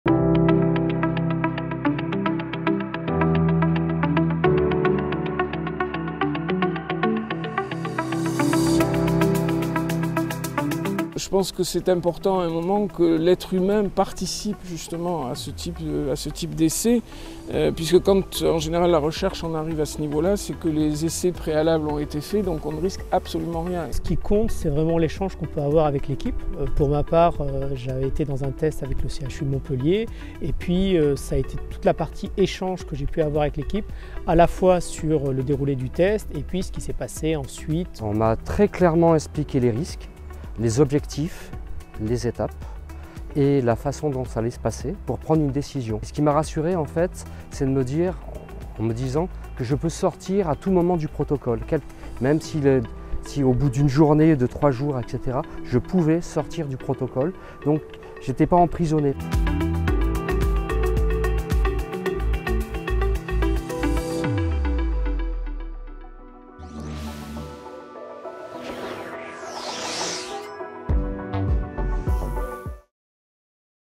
Préjugés de la Recherche : je participe à un essai clinique, je prends de grands risques
La parole est aux patients !

Déconstruire les idées reçues pour participer à la recherche et devenir acteur de la médecine de demain !

Les patients/ participants - partenaires de la recherche :
Sabine Petit, Stephane Taliana et Olivier Piau.